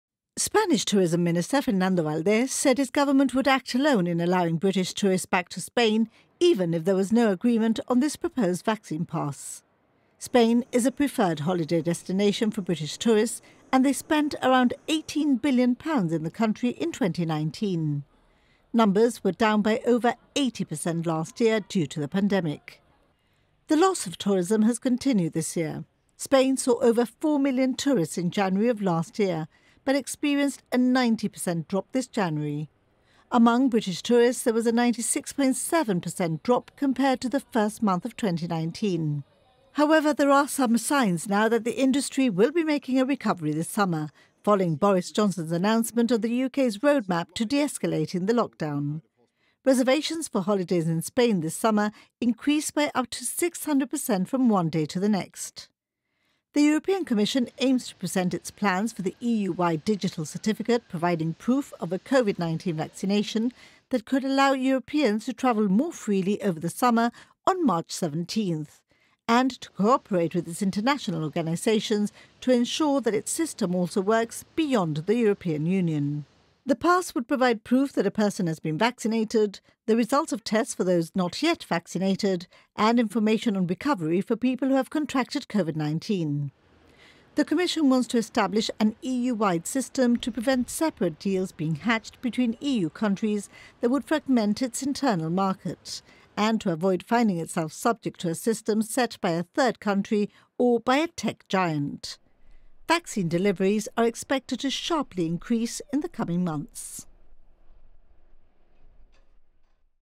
Spain and the EU plan for a return of international tourism
The President of the European Commission has announced plans for a ‘Digital Green Pass’ which could facilitate travel around Europe.

The pass would serve as proof that an individual has immunity from COVID, as well as contain information on recent test results.

Commission president Ursula Von der Leyen says the aim is to gradually enable people to move safely in the European Union or abroad - for work or tourism.